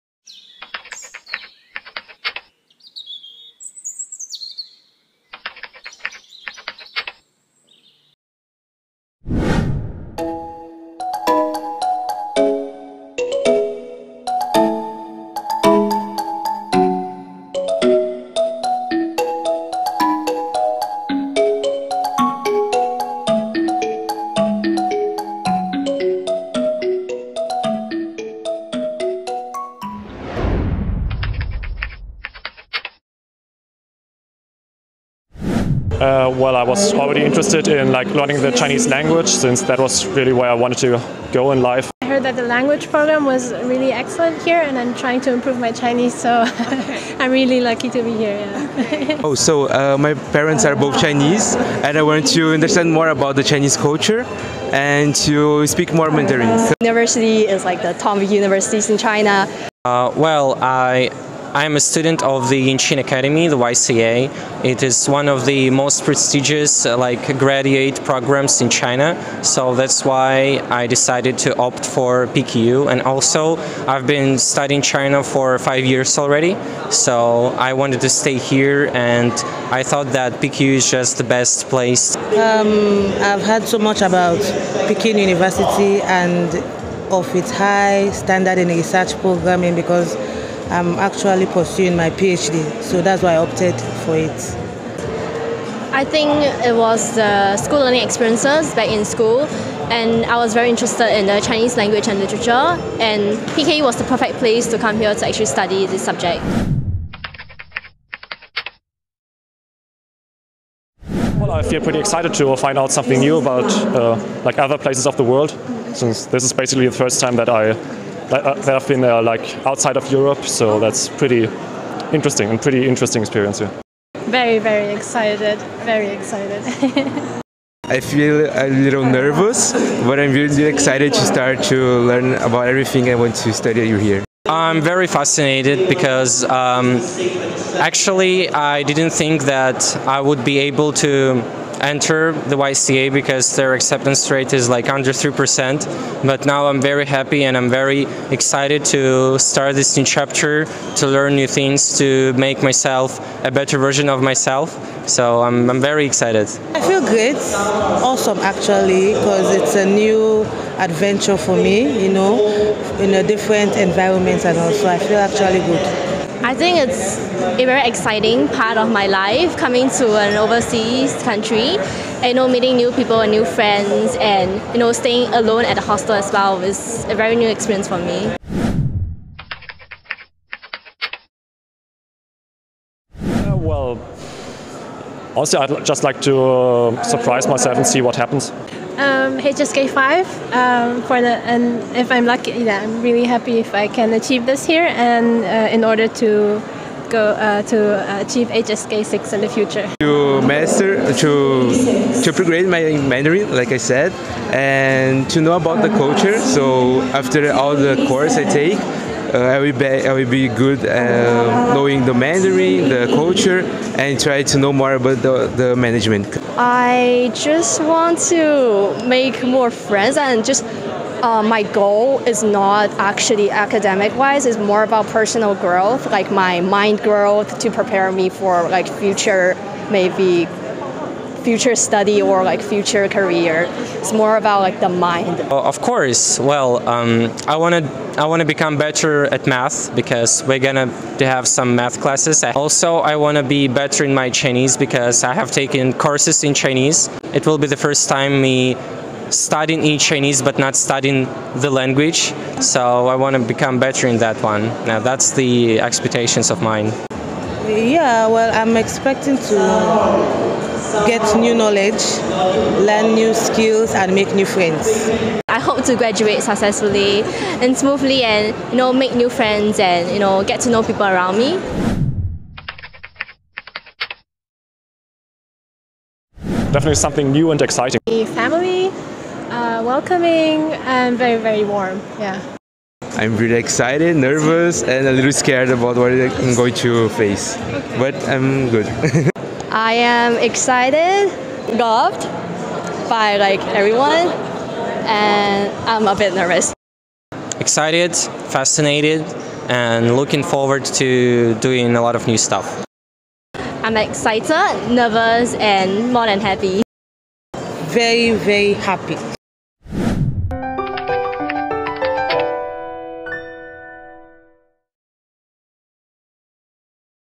Meet Fresh Faces on Move-in Day at PKU
🌍📚This September, PKU welcomes its latest cohort of internationalstudents.

Entering an unfamiliar environment, many grapple with a mix of anticipation and uncertainty. Whether outgoing or nervous, all share a gleam of excitement in their eyes as they look forward to a transformative chapter at PKU.

Watch the video to hear what our new Pekingers have to say about this new journey!🎙🍂

WeChat: PKU_Eng